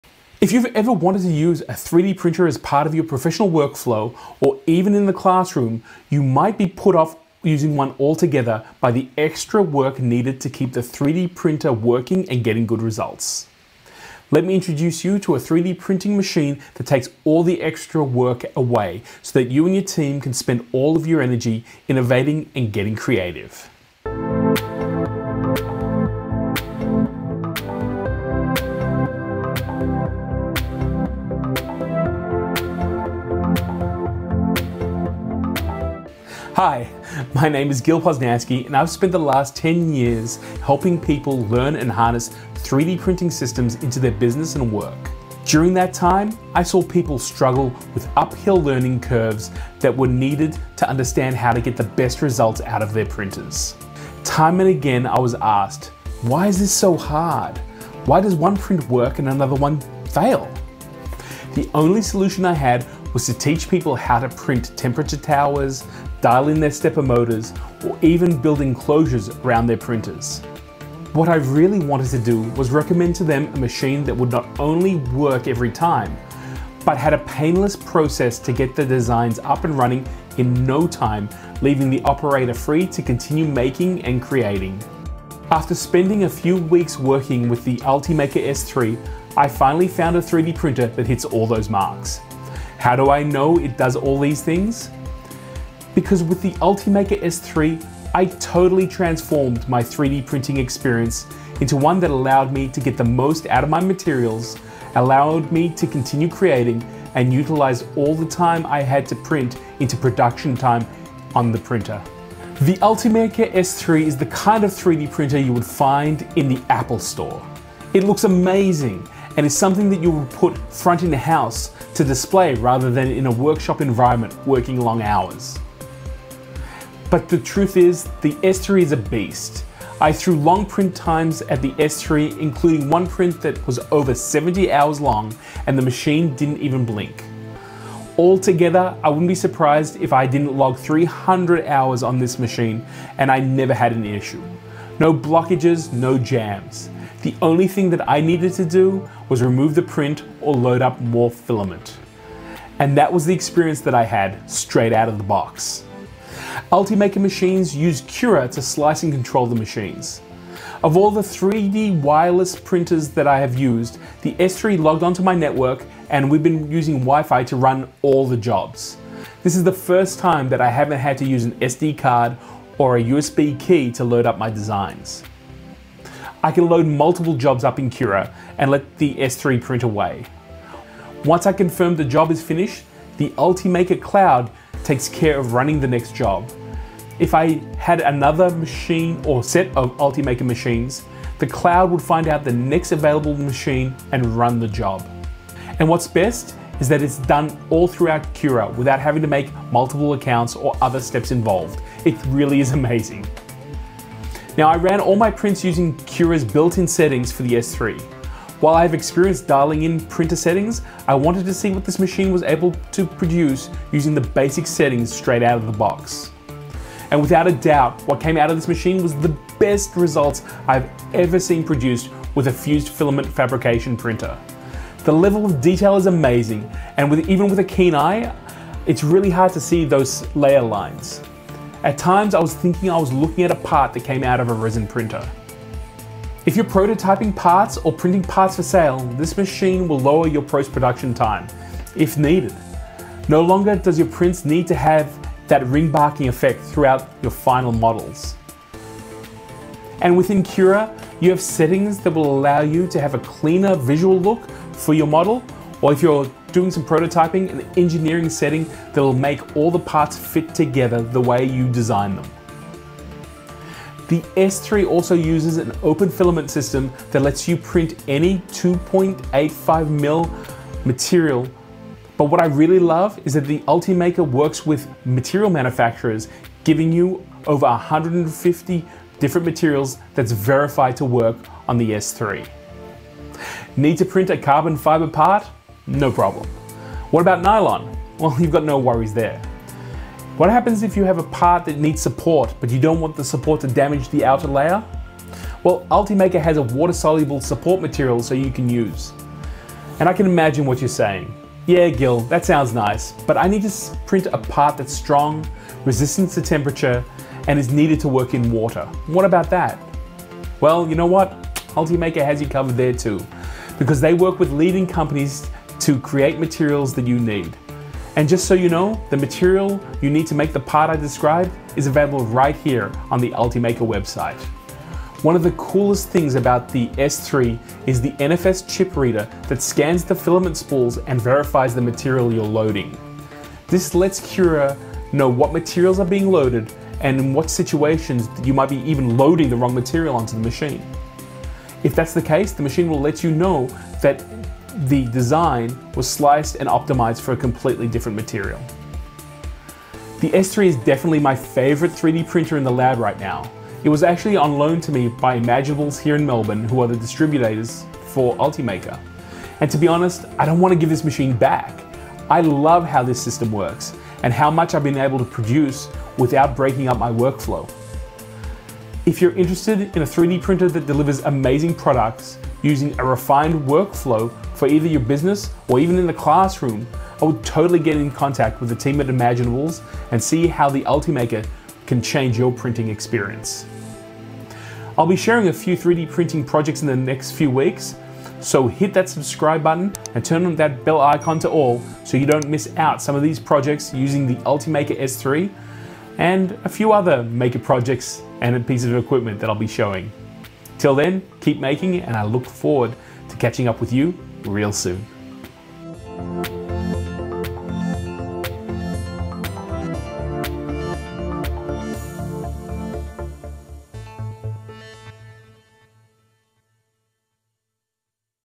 First look: Ultimaker S3 Review **In-depth Review**
Gil was able to have a first look with the Ultimaker S3 3D printer to shakedown and review.

The Ultimaker S3 is the little brother to the groundbreaking Ultimaker S5 printer, utilising the same S Series technology in a smaller and more compact model.

The Ultimaker S3 is a top of the line fused filament fabrication 3D printer that not only cleans up your workflow but produces finished results that are out of this world.

The Ultimaker S3 is Gil's favourite 3D printer in the workshop, and it looks great too! It was a pleasure to use and review this 3D printer

Big thanks to Imaginables for allowing us to review the Ultimaker S3. If you are interested in exploring the S3 further, you can contact Imaginables

00:00 - Introducing the Ultimaker S3
00:36 - Issues people find with 3D printing
01:11 - Ultimaker S3 as an answer to 3D printing problems
01:36 - Ultimaker S3 transforming workflow experiences
01:52 - Does the Ultimaker S3 belong in the Apple Store?
02:08 - Ultimaker S3 has the grunt to make it a 3D printing workhorse
02:36 - Cura and S3 make using the system simple and integrated
02:48 - Integrated Wifi or Ethernet connection
02:57 - Printing management controlled by Ultimaker Cloud integrated into Cura
03:24 - Built-in settings for printing in Cura
03:55 - Quality print results using preloaded or manual settings
04:23 - Use any 2.85mm open-source filament in the Ultimaker S3
04:55 - Ultimaker partnership with material designers for the Ultimaker S3
05:25 - NFS chip reader inside the filament spool checks what material you are using
05:50 - Gil's favourite 3D printer in the lab is the Ultimaker S3

★ If you love this review, you can share the love by hitting the "Applaude" button and giving me a tip. :)